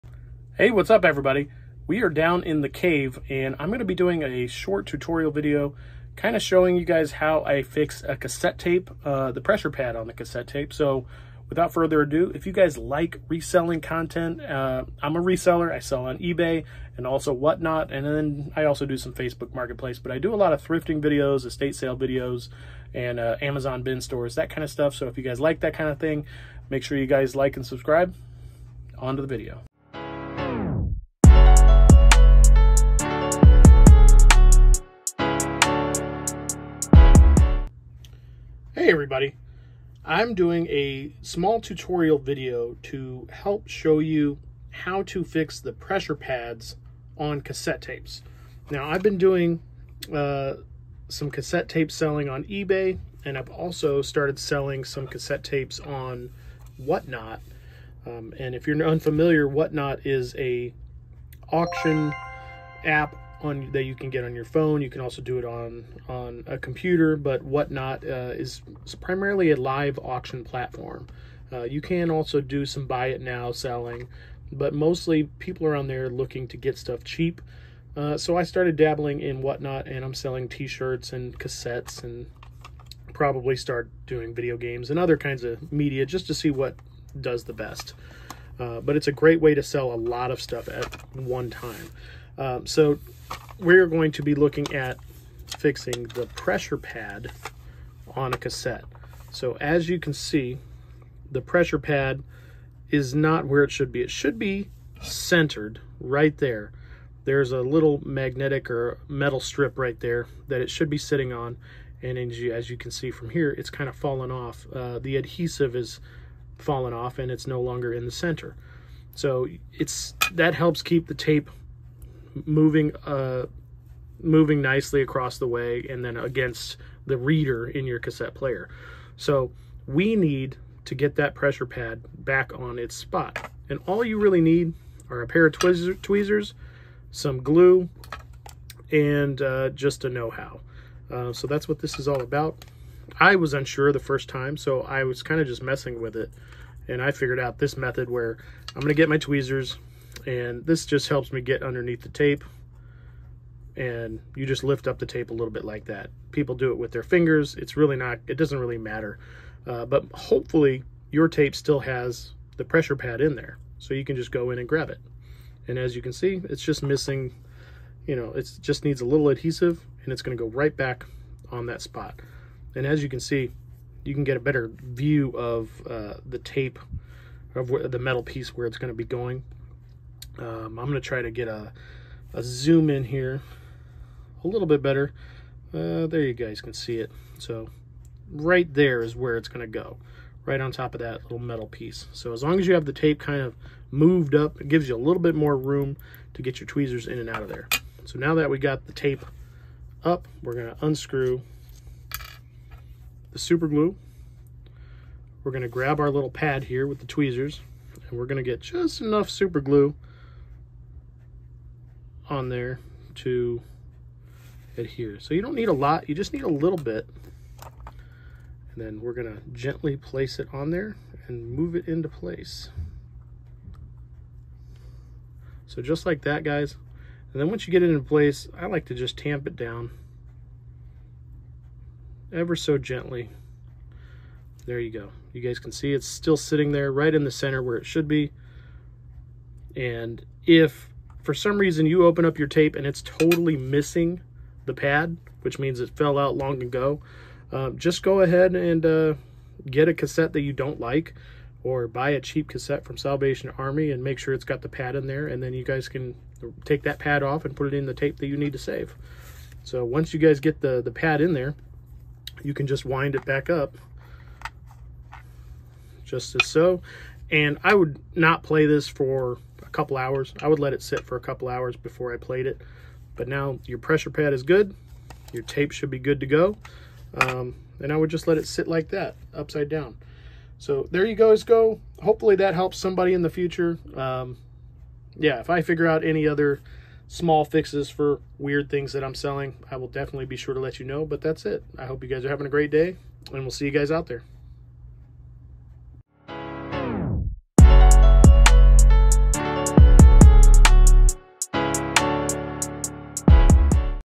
How To Fix The Pressure Pad On Your Cassette Tapes
Adding this video to help others. I have been selling cassette tapes and when you find them at sales, this occurs to quite a few of them.

If you are interested in opening a WhatNot account use this link to get a free $10 credit after you make a purchase.